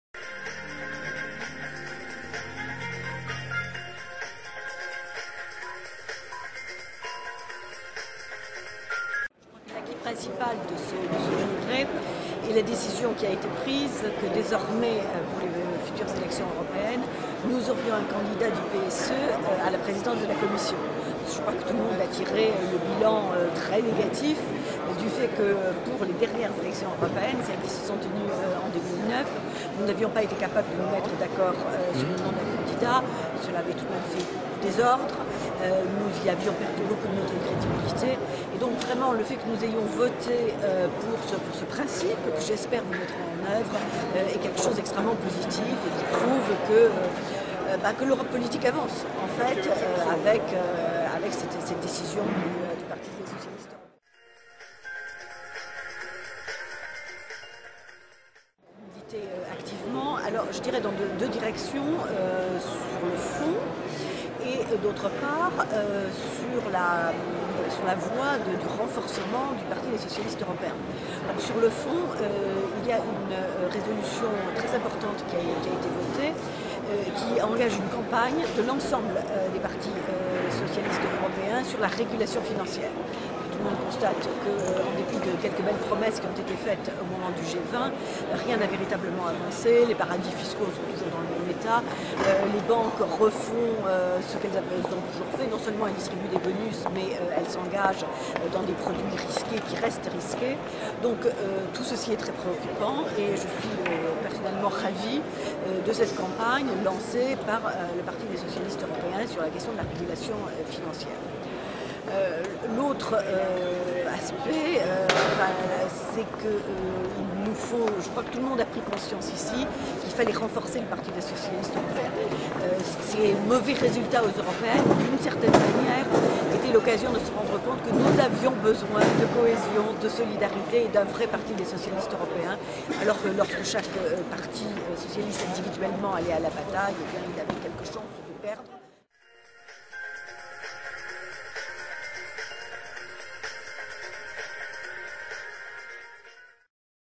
Rencontre avec Monique Saliou déléguée du PS
Rencontre avec Monique Saliou, déléguée du PS, commission fédérale Europe de Paris lors du congrès du PSE à Prague
Réupload du 12/01/10